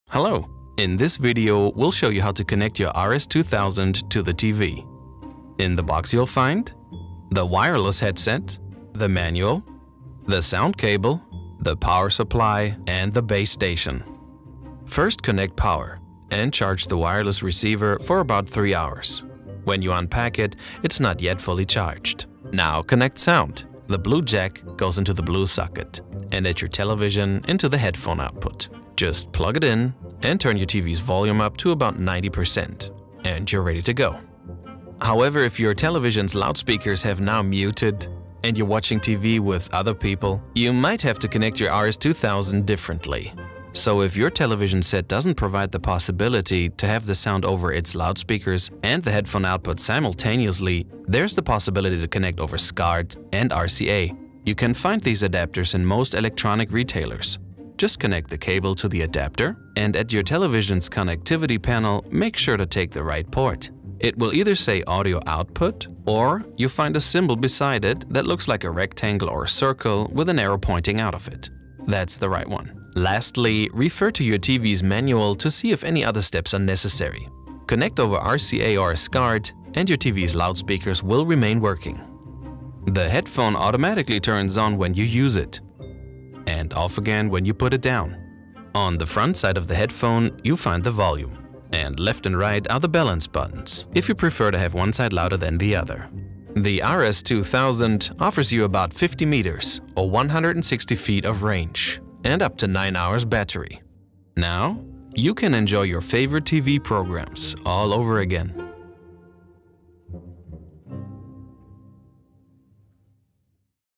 Tutorial: How to connect your Sennheiser RS 2000 to your TV I Sennheiser
This tutorial shows you how to connect your Sennheiser RS 2000 to your television.

A short introduction to the product`s features is also included.

You`ll be all set and good to go in a few minutes!

Sennheiser – Shape the future of audio!